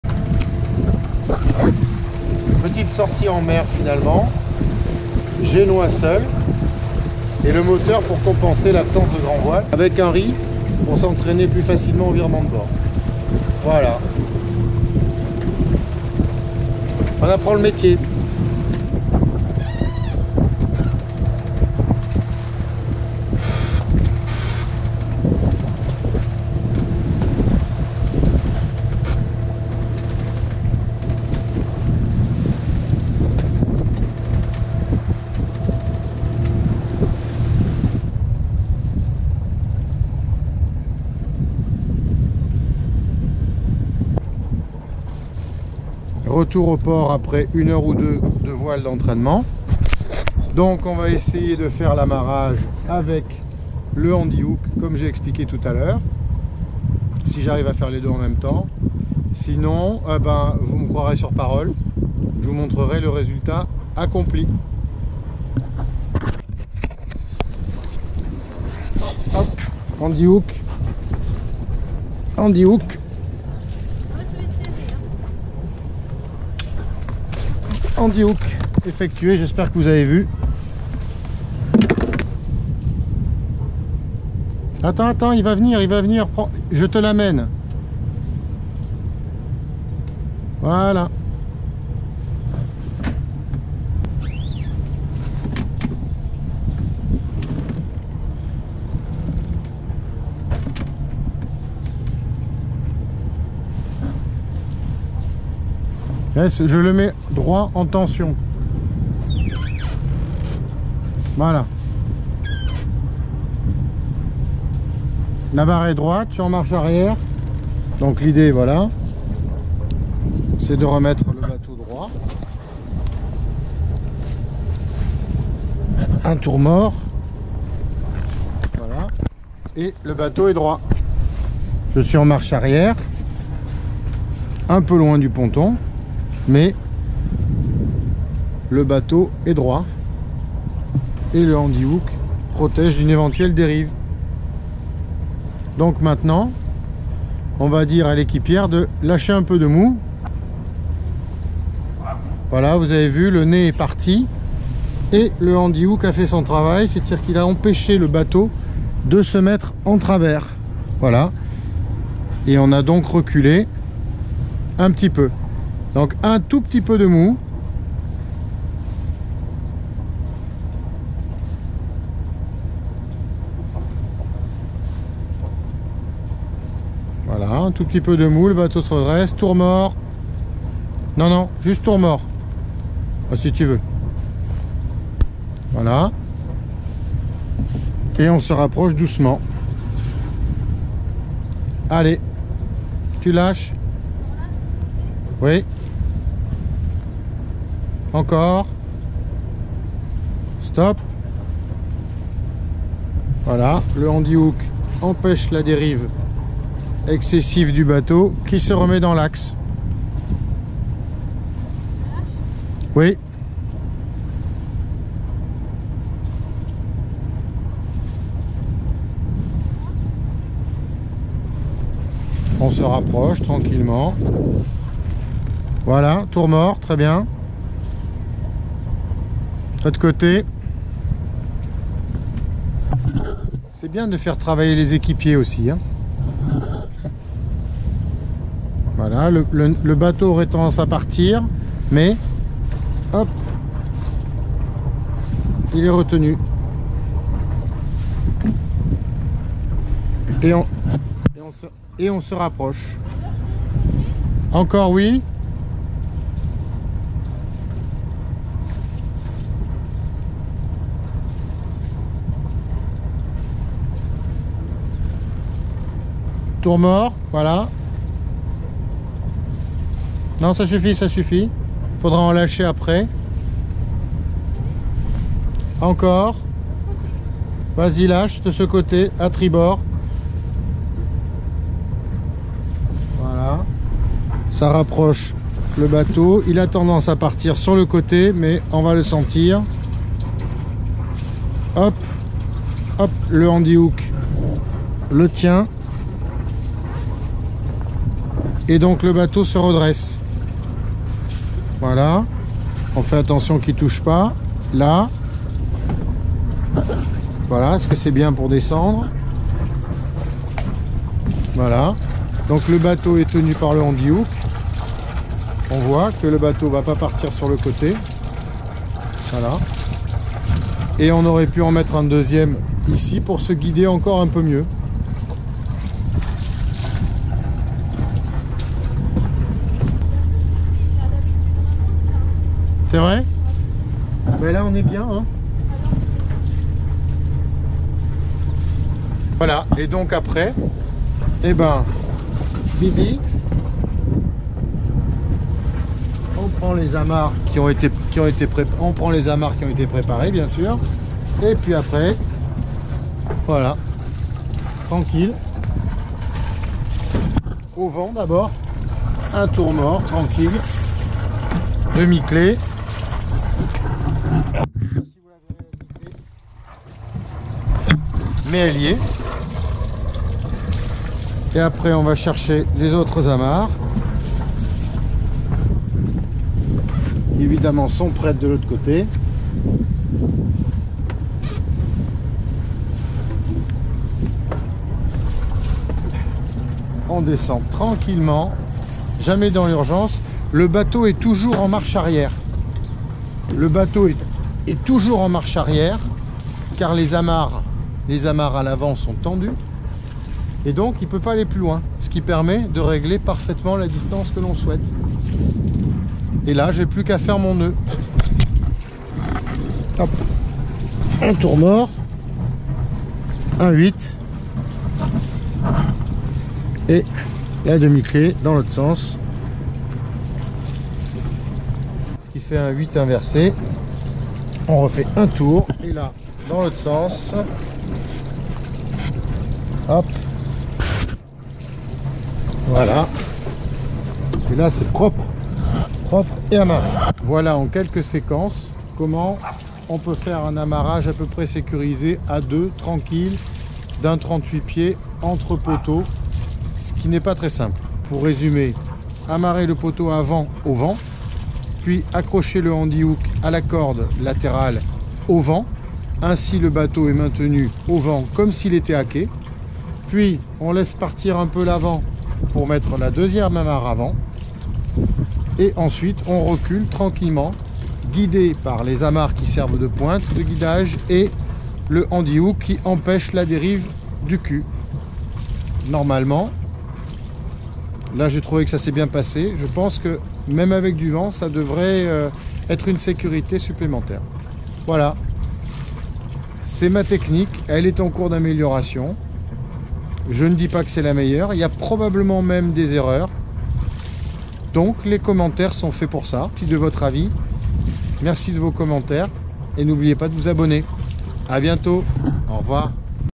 HANDY DOCK DERNIER VOLET DE TEST GRATUIT SANS CASHBACK DE WISH OU DE LA MARQUE
Dans ces vidéos sur le produit HANDYDOCK je ne donne que mon avis sans sponsor ni intérêt commercial , j'ai simplement trouvé ce système sécurisant pour gérer l'amarrage avec "ducs d'Albes", ce qui est loin d'être évident en équipage réduit .
Depuis j'ai amélioré ma technique en passant le bout au winch qui me permet de remonter le bateau au vent très facilement , j'envisage d'adapter la technique avec un autre HANDYDOCK pour amarrer l'avant de manière transitoire et ainsi tenter la manœuvre en solo ( il va vraiment falloir que je contact la marque ;) )
à suivre ….